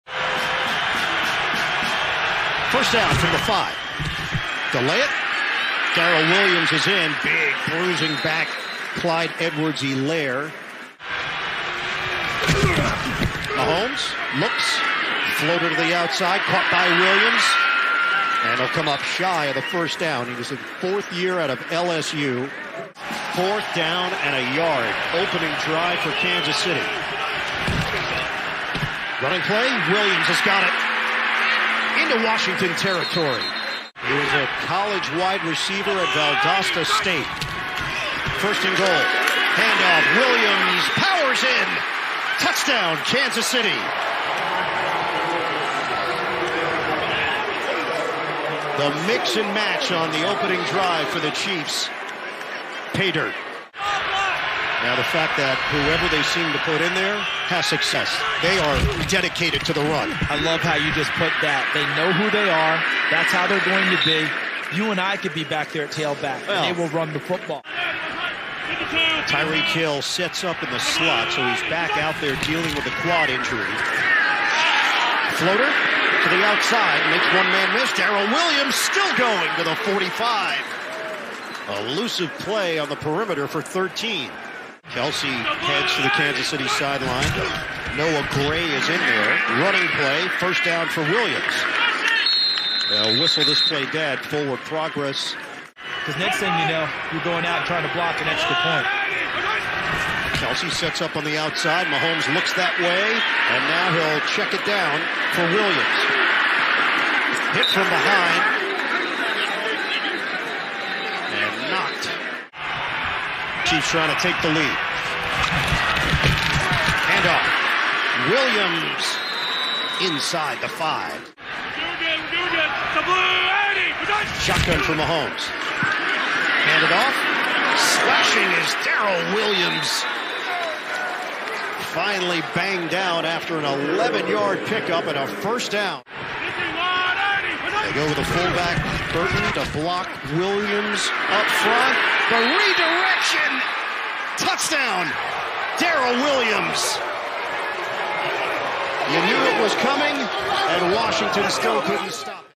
Darrel Williams' Best Plays from Week 6 | Chiefs vs. Washington
Check out Darrel WIlliams' best plays from the Week 6 matchup against the Washington Football Team.